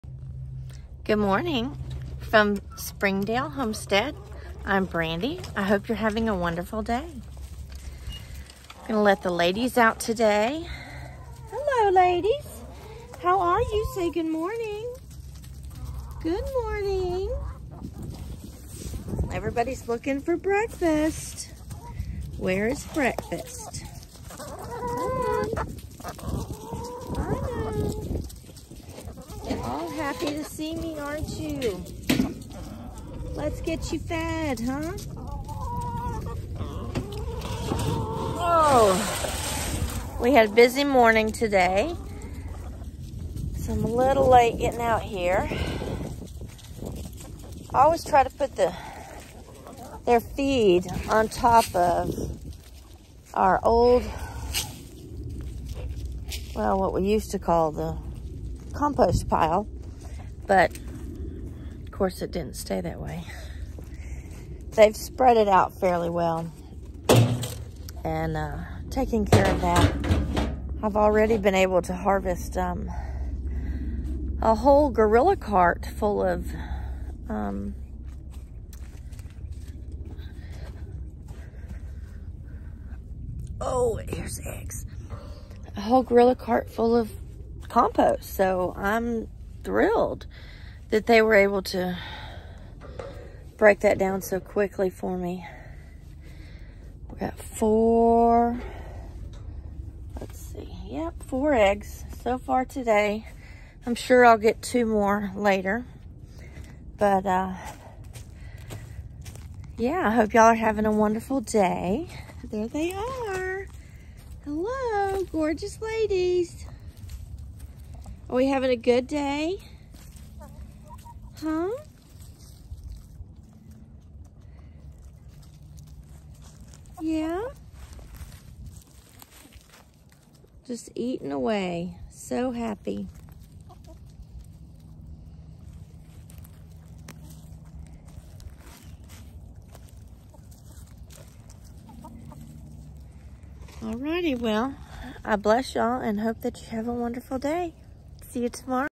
Waiting on me, as usual!
Chicken breakfast on the compost pile.